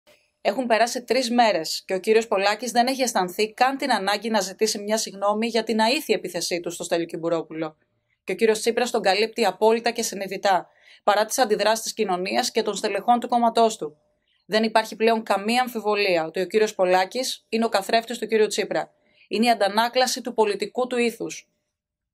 Δήλωση Σοφίας Ζαχαράκη για την ταύτιση του κ. Τσίπρα με τον κ. Πολάκη
Δήλωση της Εκπροσώπου Τύπου της Ν.Δ. κ. Σοφίας Ζαχαράκη για την ταύτιση του κ. Τσίπρα με τον κ. Πολάκη.